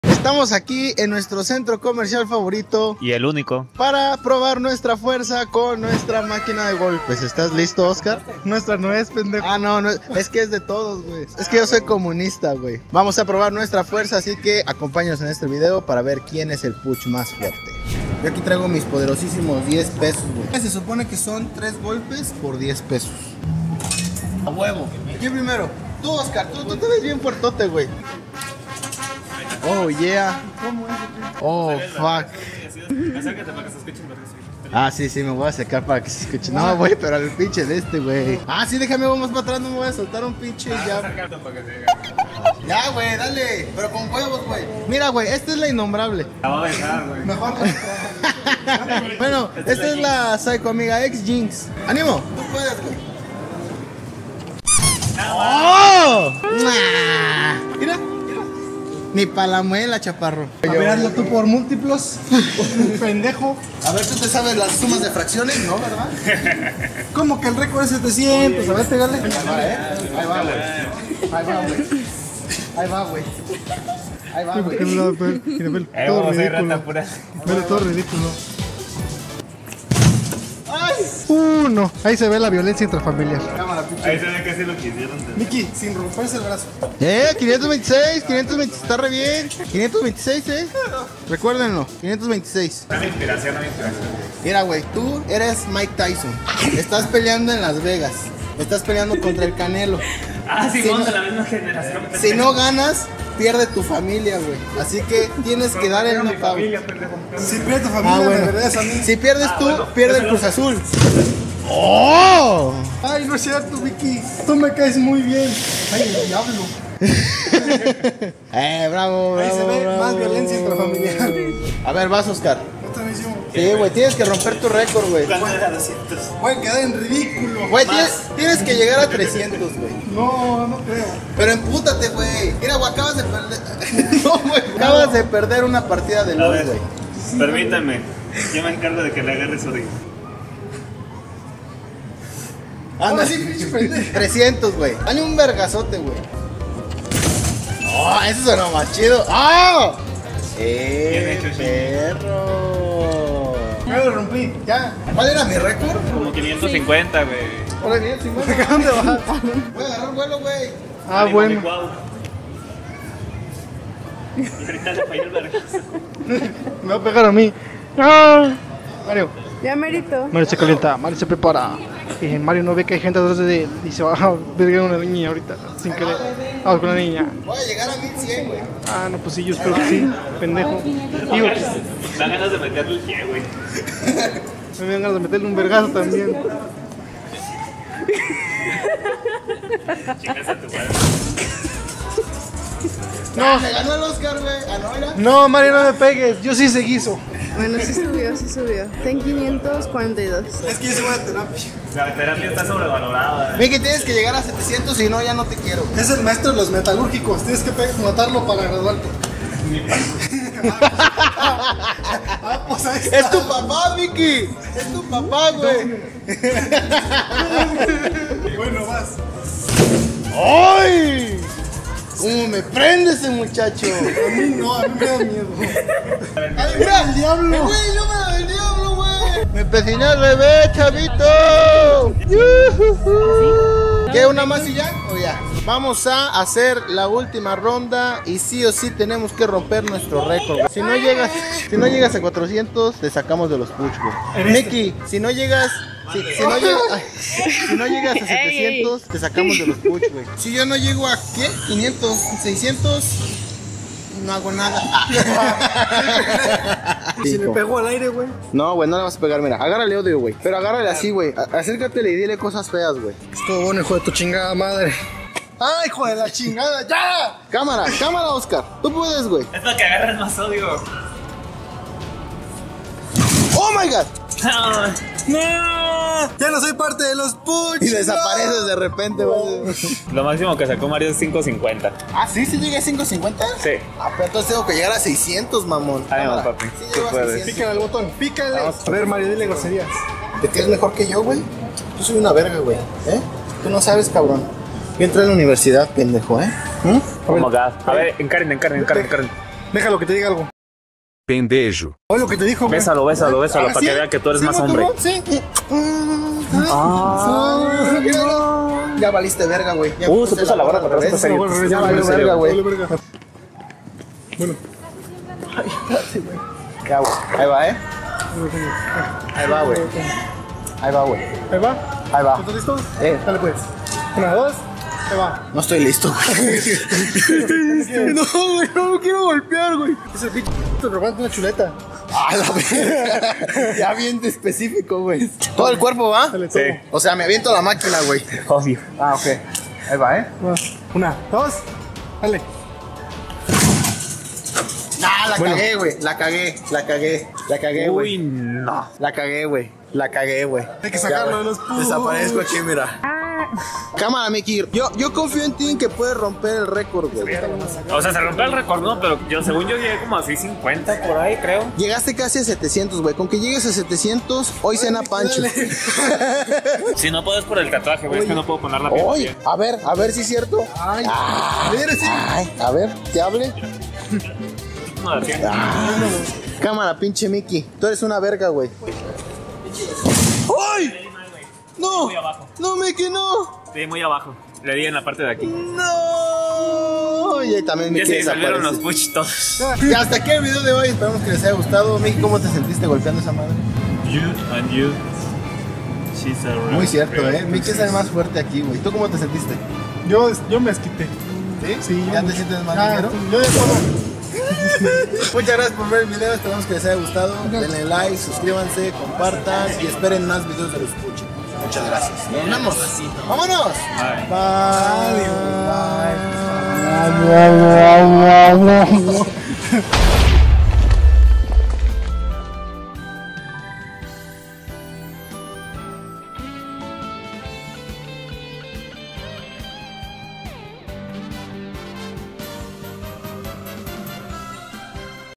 LOS PUCH vs MAQUINA DE BOXEO
Buenas noches, niños y niñas. bienvenidos a un nuevo video en el canal de Los Puch
En este video Los Puch se enfrentan a una maquina de boxeo para saber QUIEN ES EL PUCH MAS FUERTE...